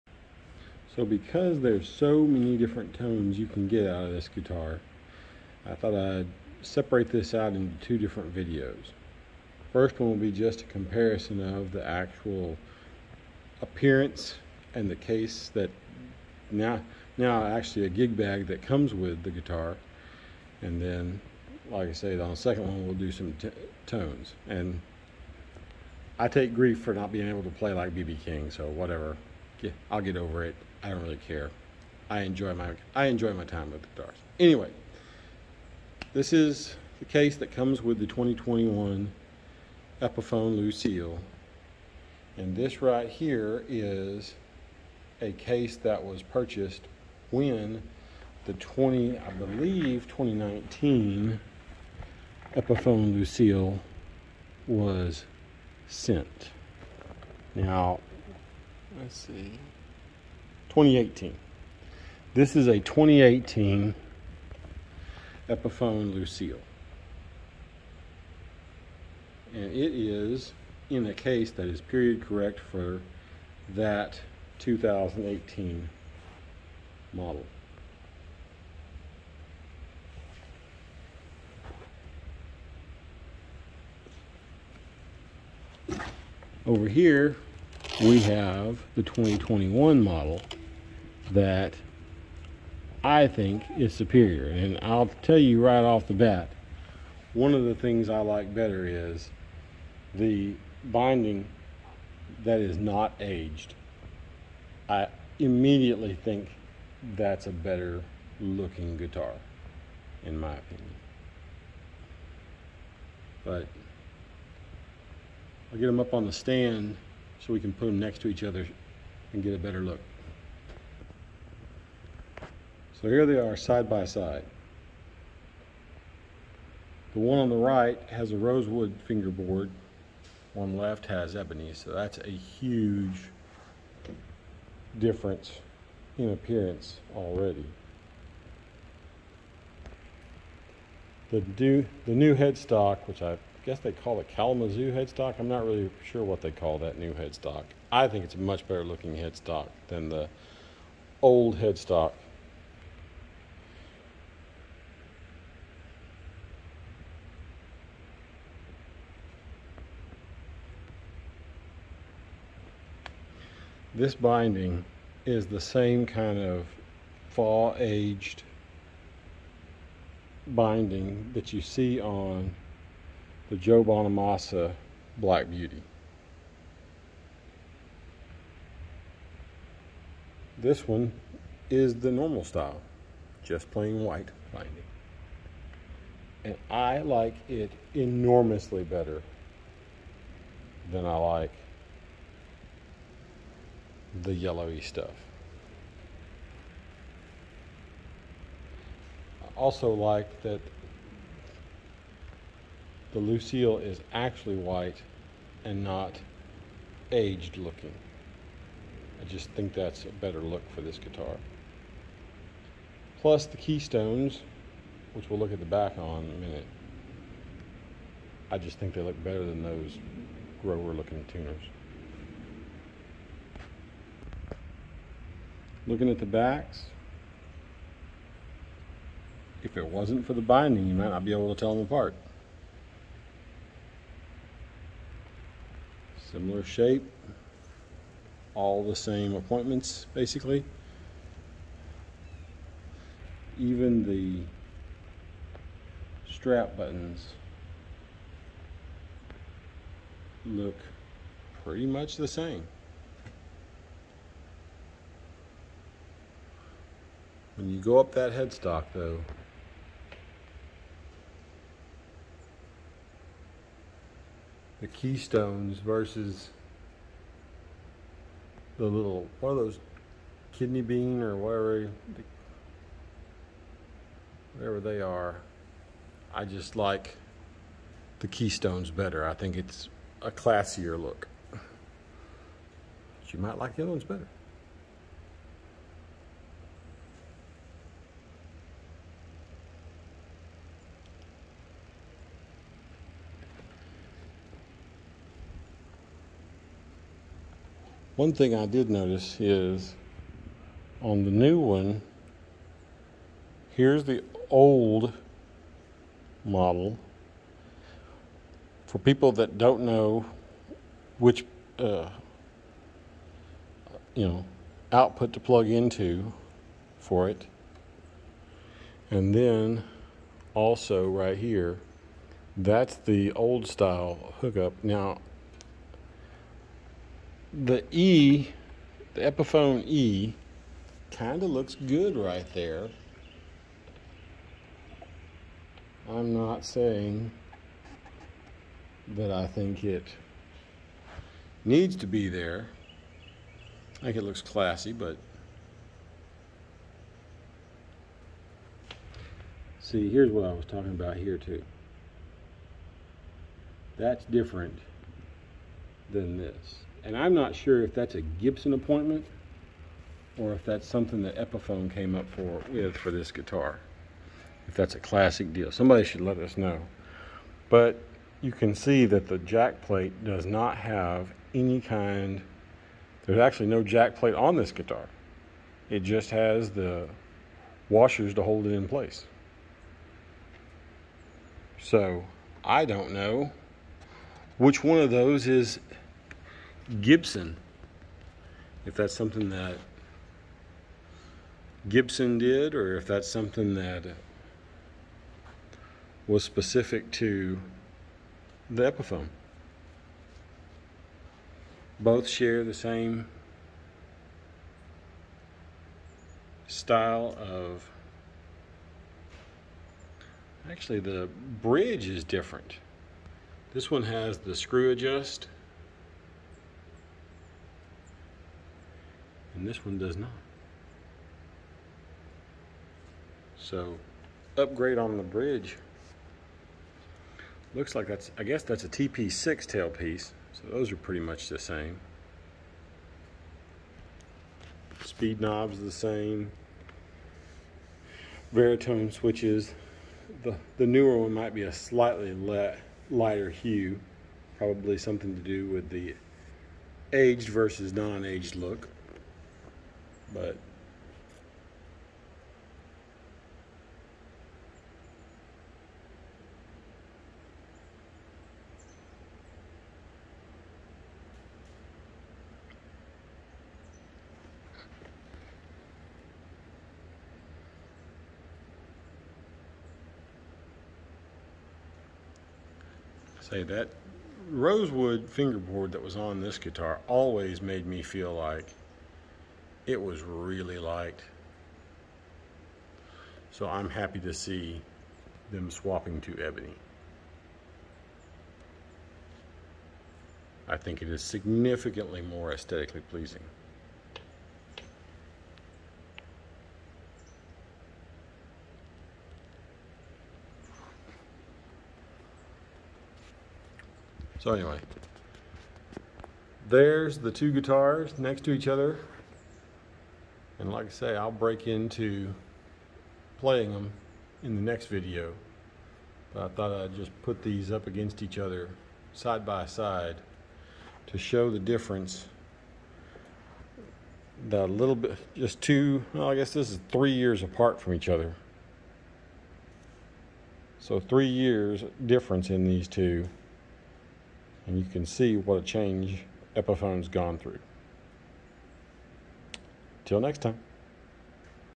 2021 Epiphone Lucille vs 2018 Epiphone Lucille : appearance and appointments video
Comparing the 2021 Epiphone Lucille to the 2018 Epiphone Lucille.  This video is all about the appointments and appearance of the two models.  There's a lot that has been changed for the 2021 model Lucille.  Which one do you like better?